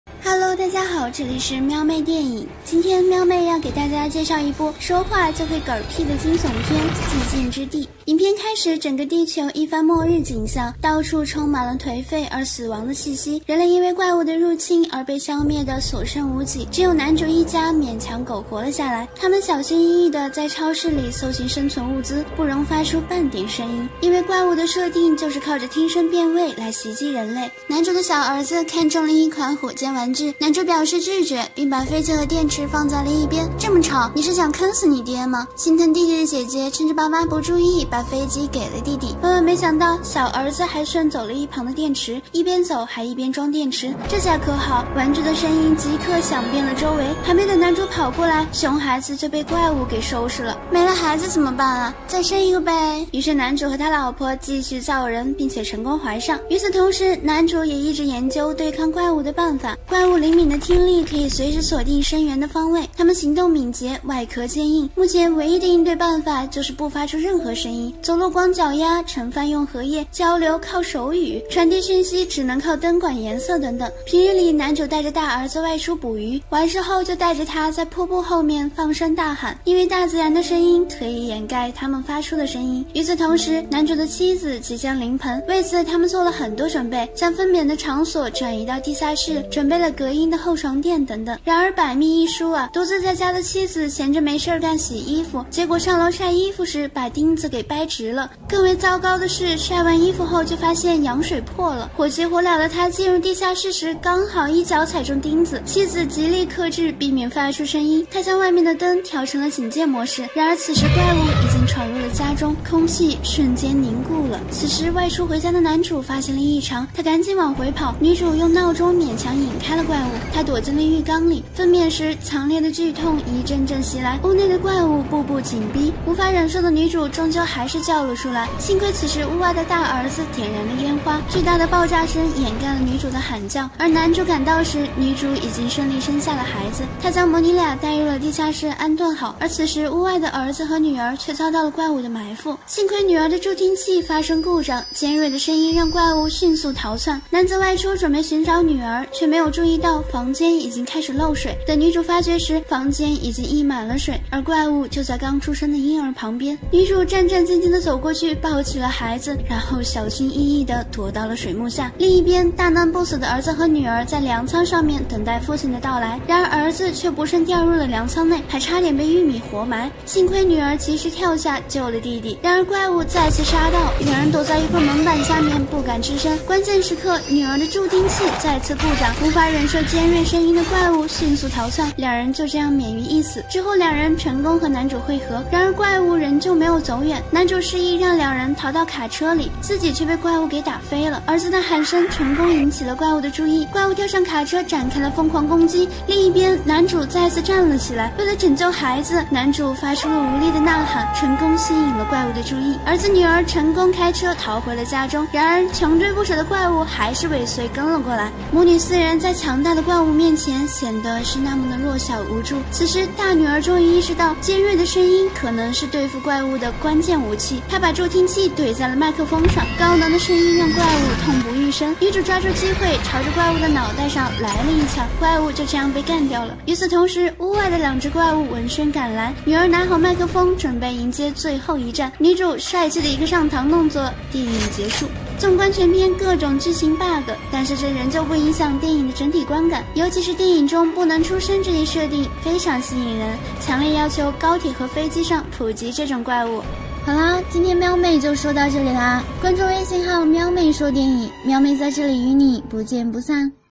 神秘怪物拥有超灵敏耳朵，人类一出声就被秒杀，要想活命必须安静
电影、影视、惊悚片、恐怖片